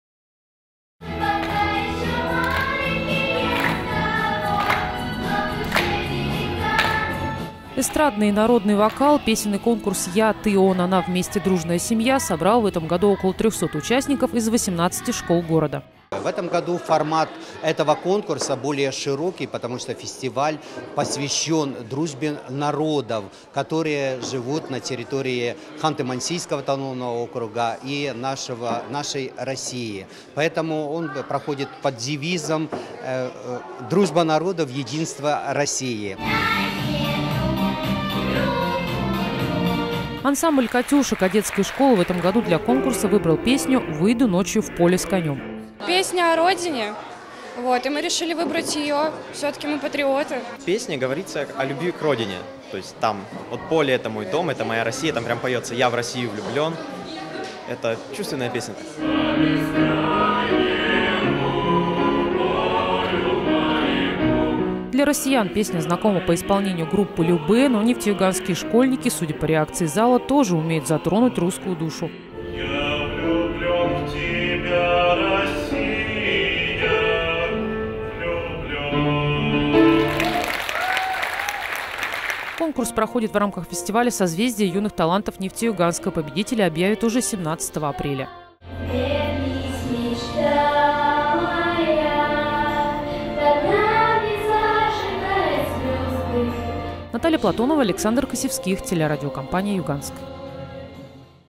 Кумыкские, татарские
От русской кадрили до кумыкской частушки. В Нефтеюганске проходит конкурс песенного творчества среди школьных ансамблей. За один день жюри услышало более 60 номеров. Некоторые песни нашим ученикам удалось спеть лучше столичных исполнителей, зрители слушали с открытым ртом.